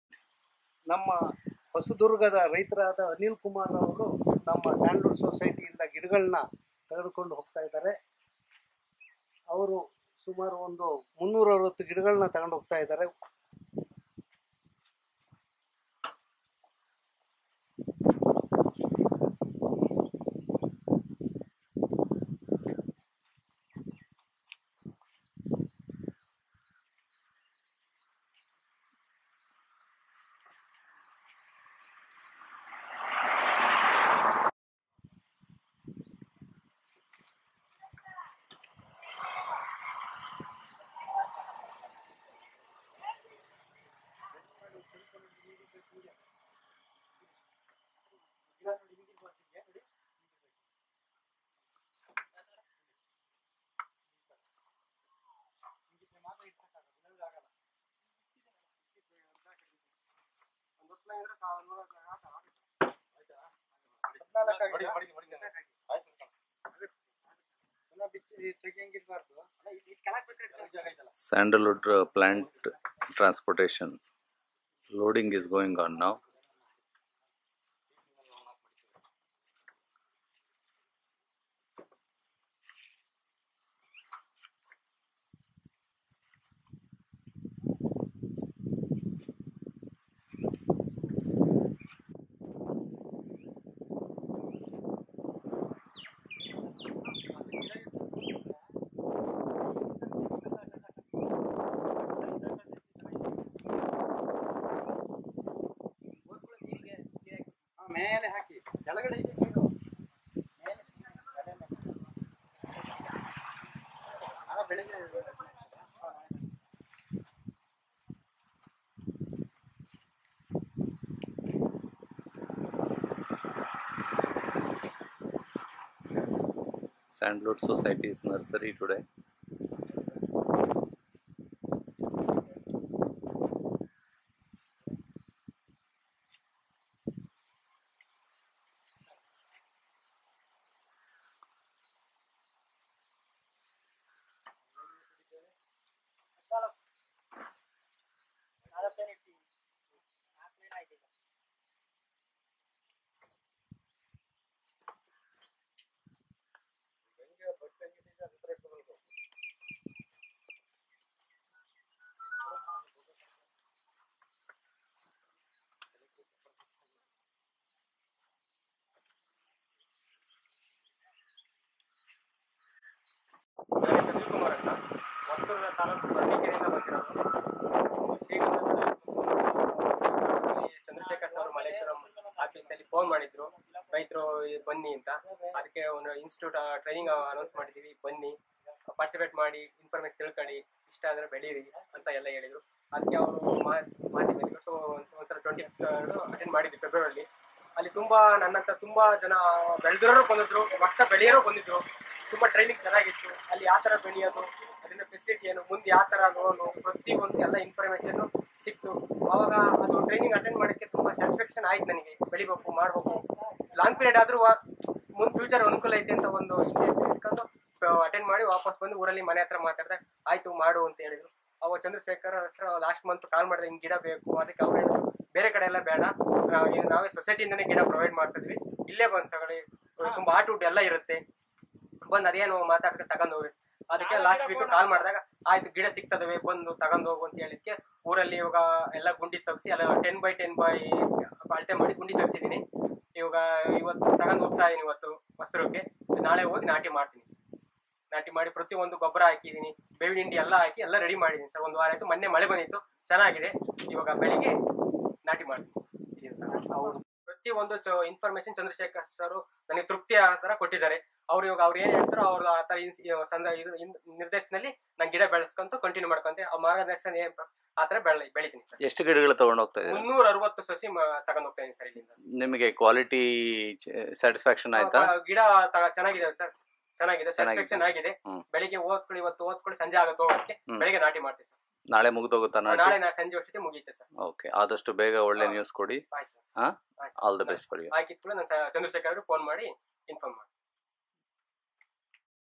High quality Sandalwood Seedlings
Selection of healthy seedlings is necessary for successful plantation. Get best sandalwood seedlings with technical advise from our society at a reasonable cost.